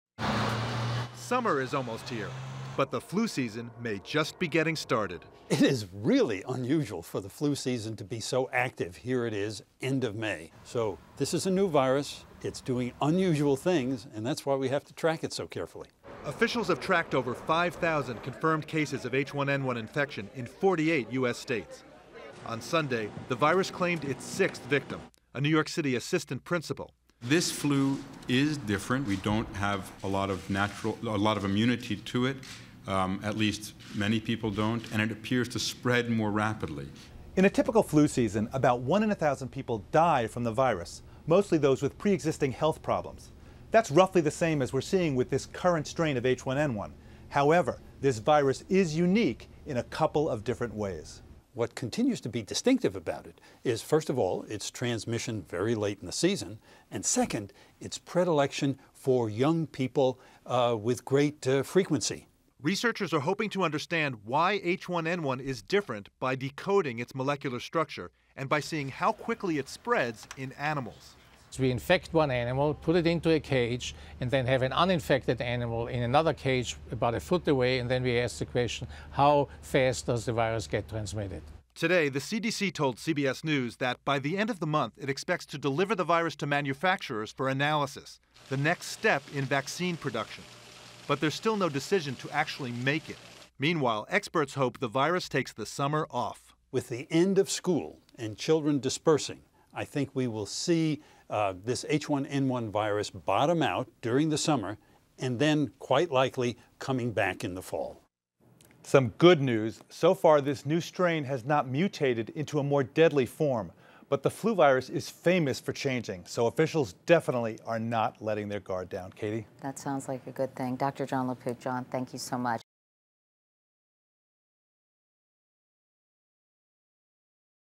H1N1's Unique Strain
In a typical flu season, about one in a thousand people die from the flu. That's about the same as what we're seeing with the current strain of H1N1. But, as Dr. LaPook reports, there are unique differences.